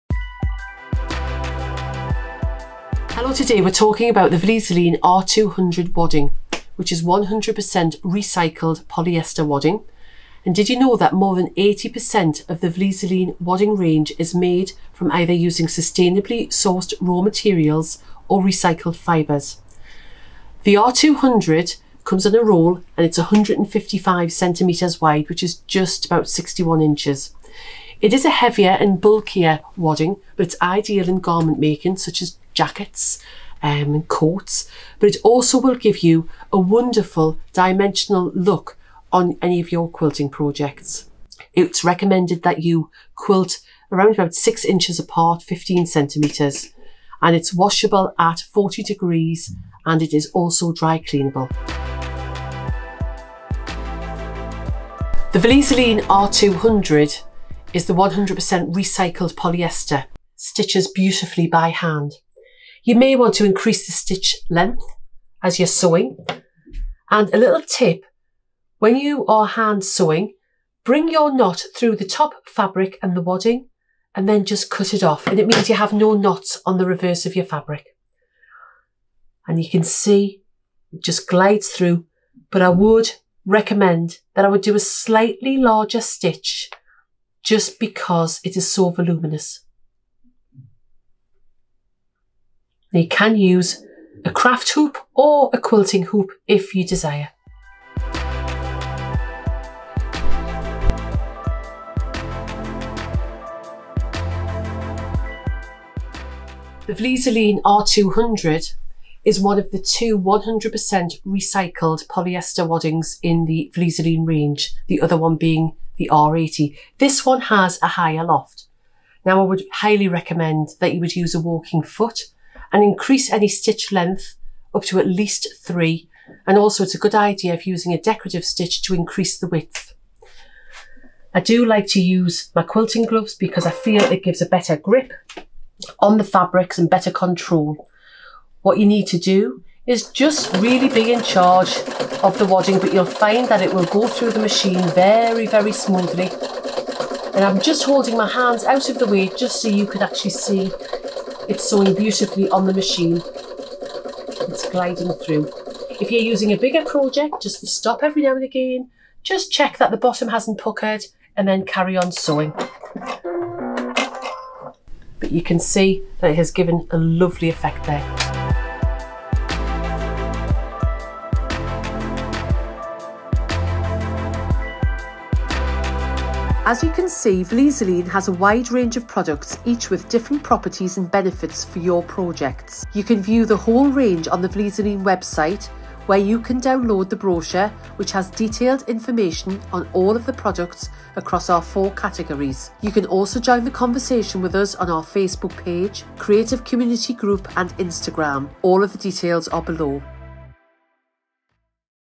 🧵 **Discover the Magic of R200 Volume Wadding!** 🧵
Calling all quilters, crafters, and sewing enthusiasts! 👋 Get ready to elevate your projects with Vlieseline's R200 Volume Wadding. This heavy-weight, bulky wadding is a game-changer for quilting, patchwork, bags, and garments.

In this quick tutorial, you'll learn:
• What makes R200 unique
• Its eco-friendly composition (100% recycled polyester!)
• How to apply and care for R200
• Tips for incorporating it into your next sewing adventure

Whether you're a beginner or a seasoned pro, this video has something for everyone. 🎨✂

🌐 Find out more:
• Vlieseline Website
• Find a Stockist

Happy sewing! 🪡💖